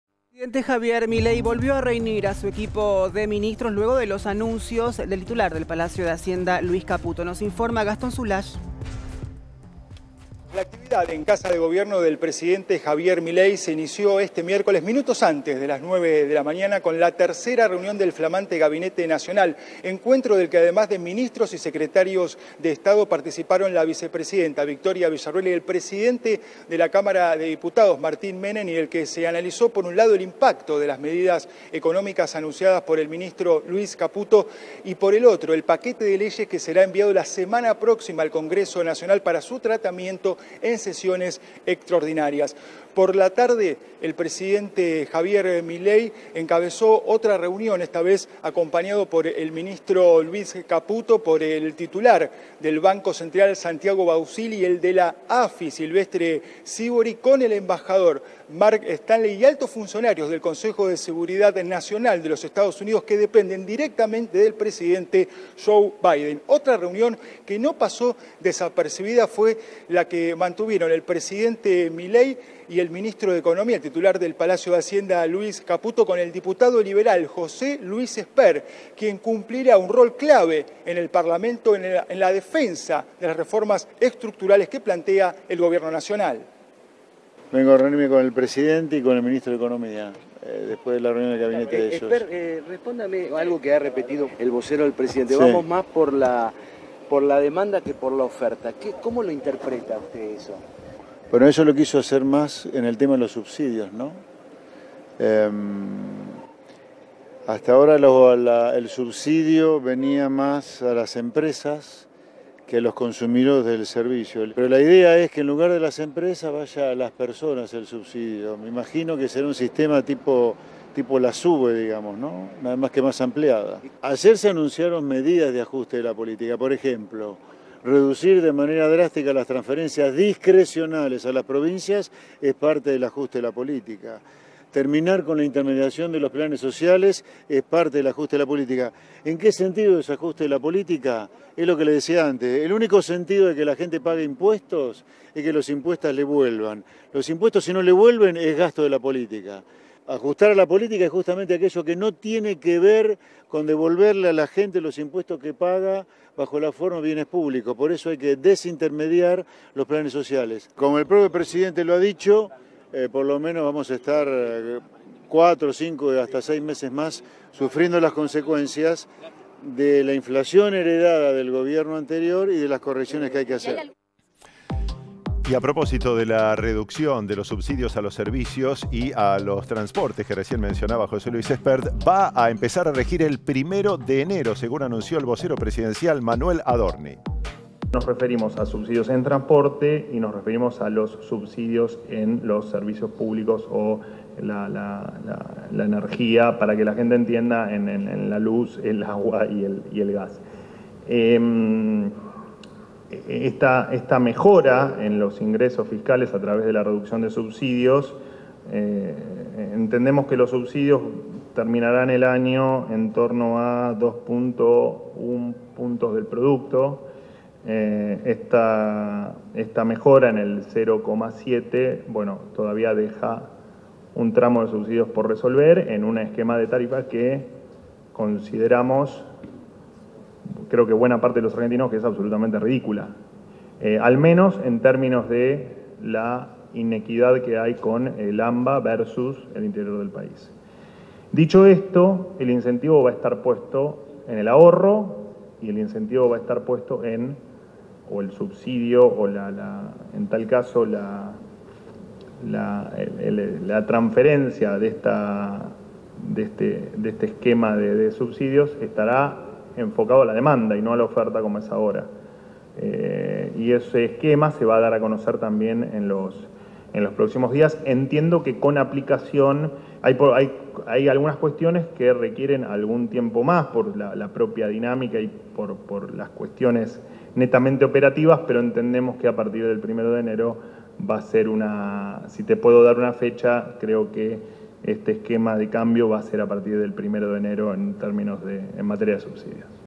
El día después del nuevo paquete económico de Caputo
En Casa Rosada se continúan realizando reuniones para afinar el plan de trabajo del nuevo Gobierno. José Luis Espert también se reunió con el Presidente y el ministro de Economía.